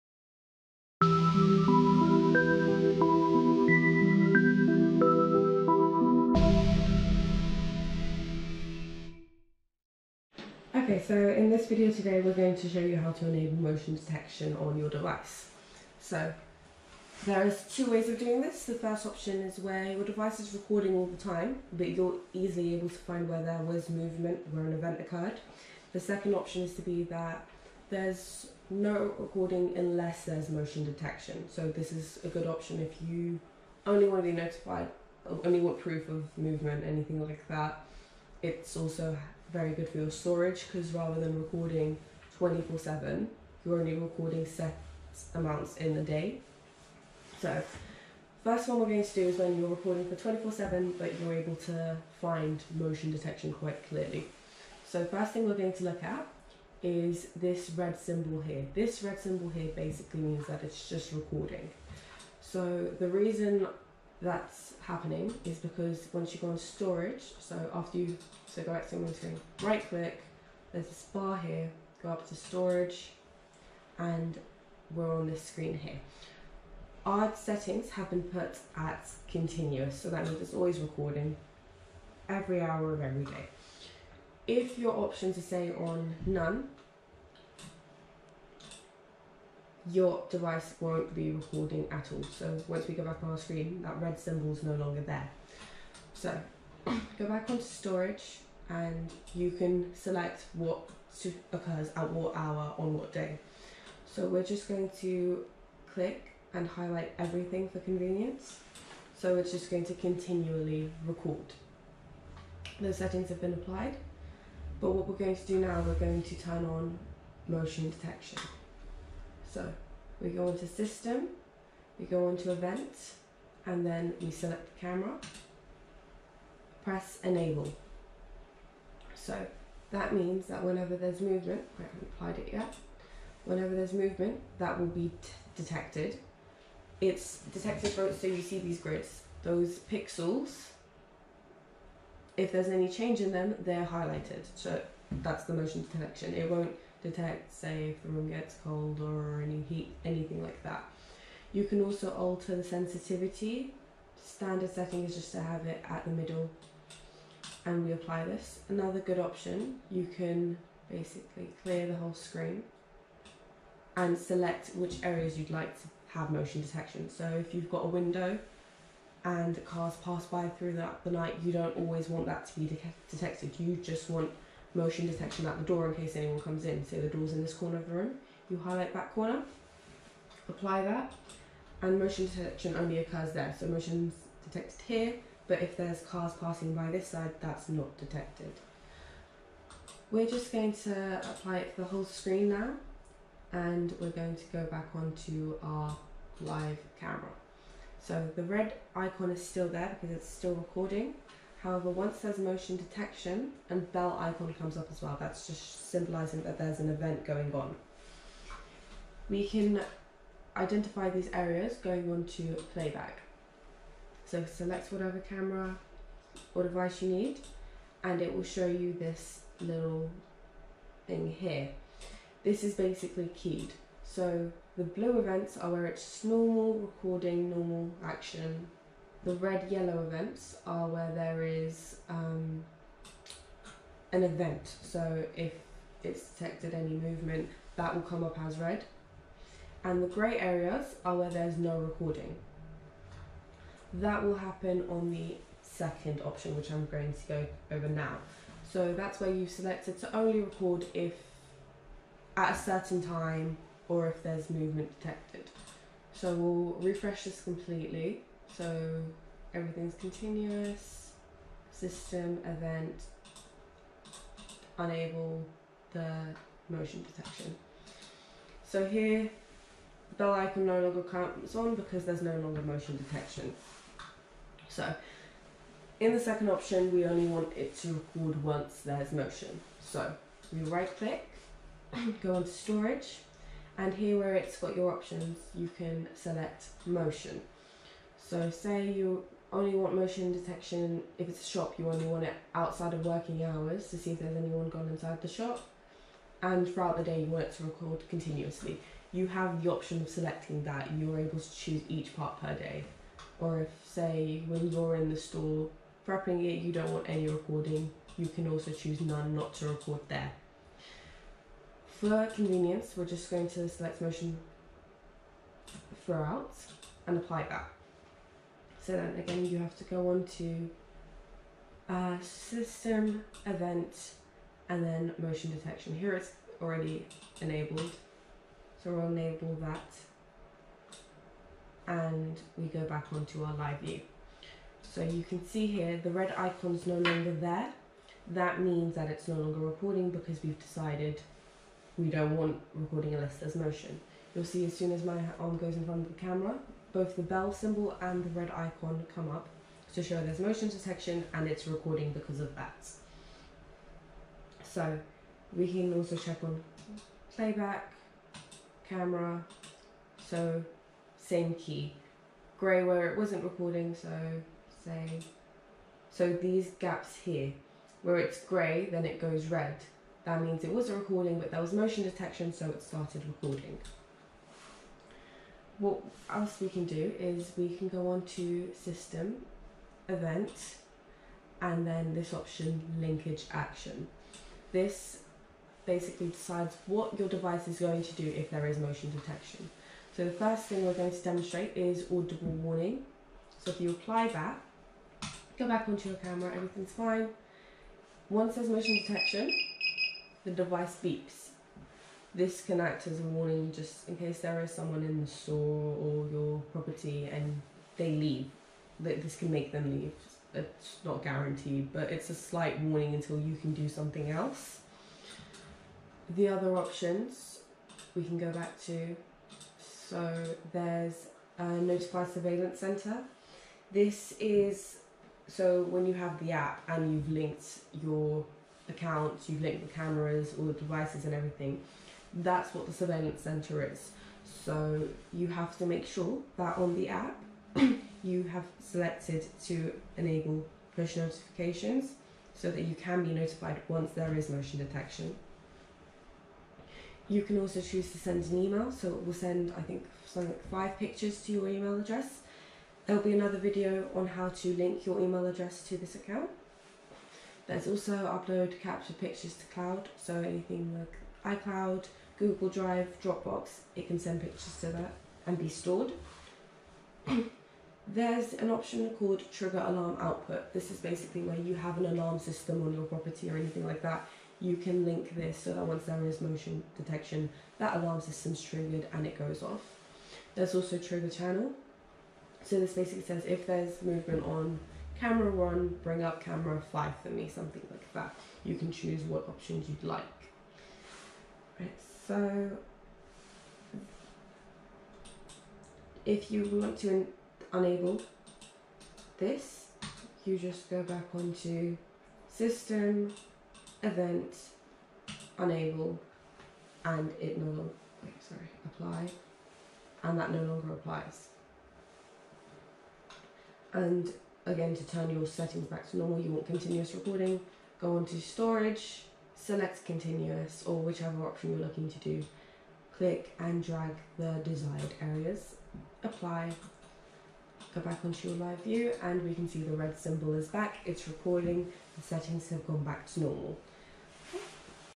HOW TO SETUP CCTV MOTION DETECTION ON HIKVISION DVR/NVR LATEST FIRMWARE (2019) TUTORIAL
In this video, we will show you how to setup motion detection on your Hikvision DVR/NVR